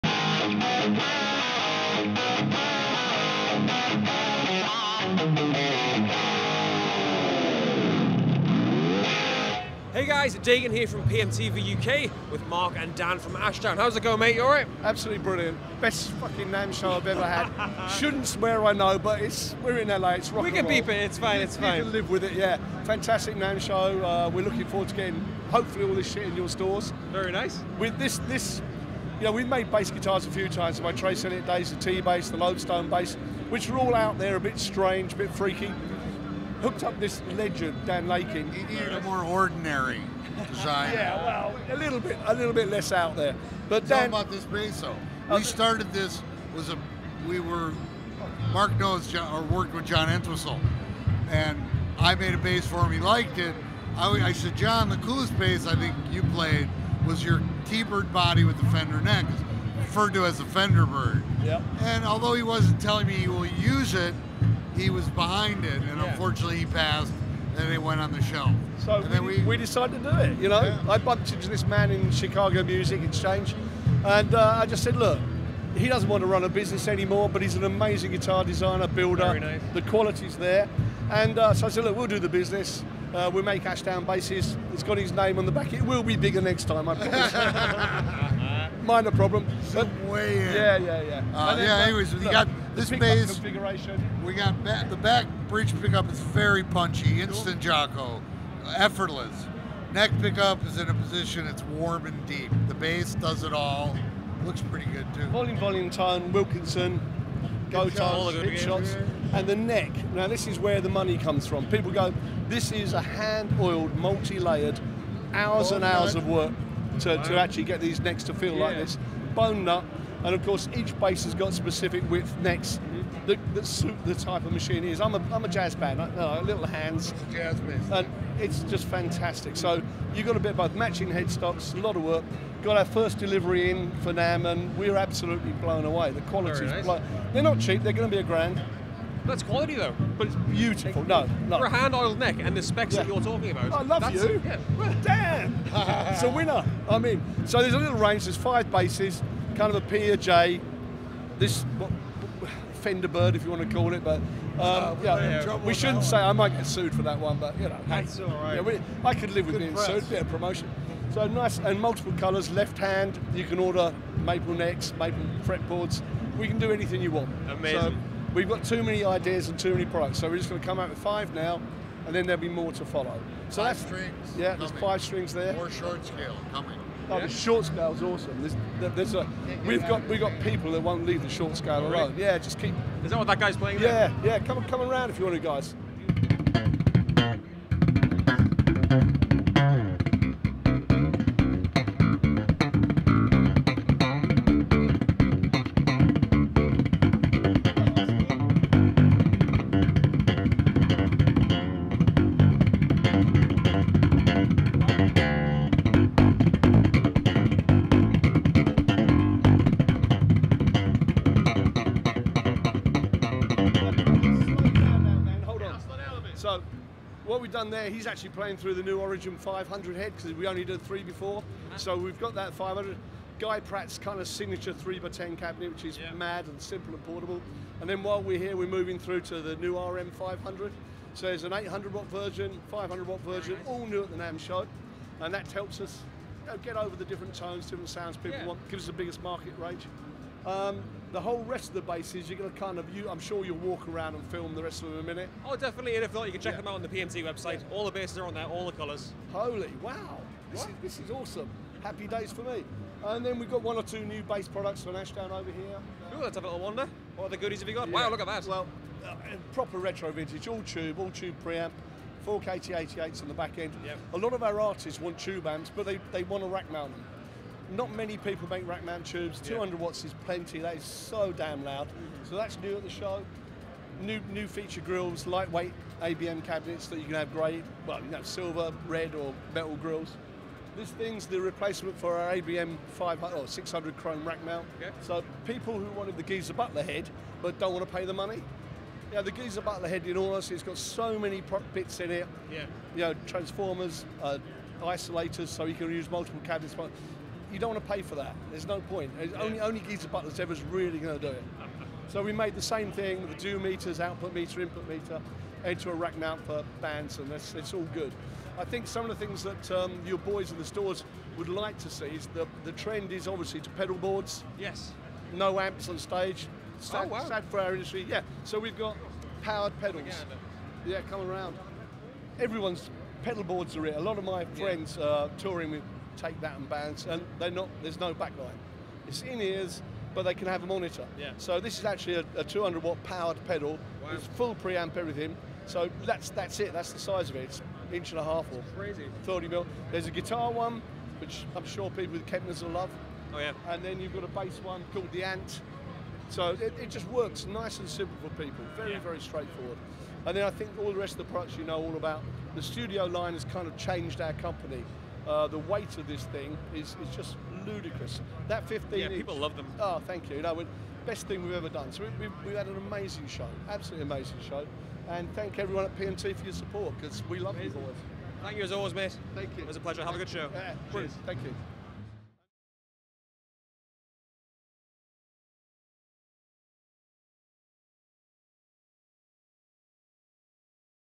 Ashdown Bass Guitars By Dan Lakin, Heads, Cabs & Pedals | NAMM 2020
Dagan checks out the new Ashdown Bass guitars designed by Dan Lakin (of Lakland basses) as well as the new heads, cabs and pedals.

Shop all the new Ashdown bass gear over at PMT

Dan Lakin is back in action, designing bass guitars for one of the coolest bass amp manufacturers around. Watch as we get a look at the new Ashdown bass guitars designed by the man behind Lakland bass guitars as well as the Origin 500 heads, RM 500, CTM Heads and more at NAMM 2020.